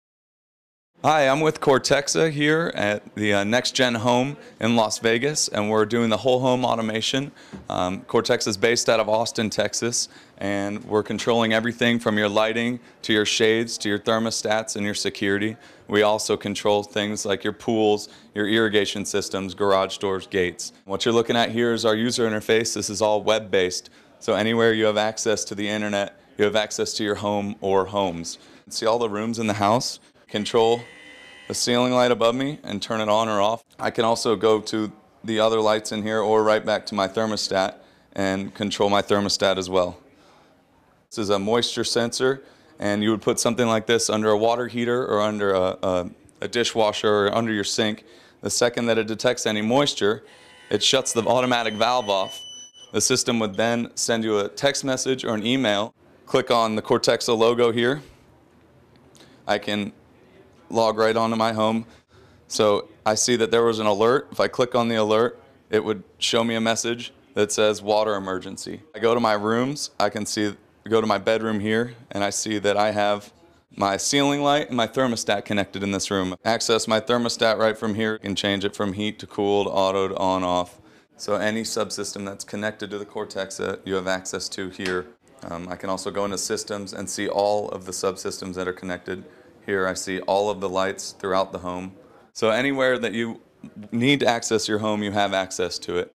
Cortexa @ IBS 2009
Demonstration of Cortexa's new intelligent home management system using the new web-based user interface and iPhone application